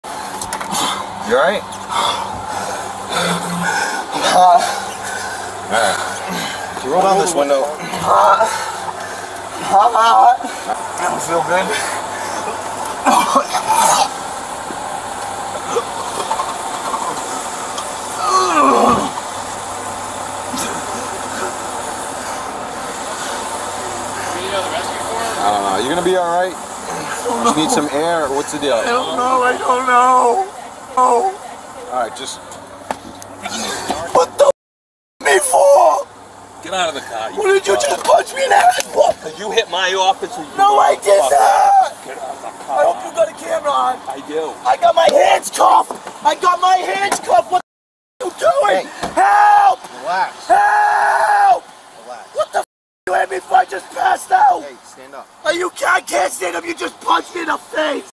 Sergeant punches handcuffed man
Cop does something he shouldnt do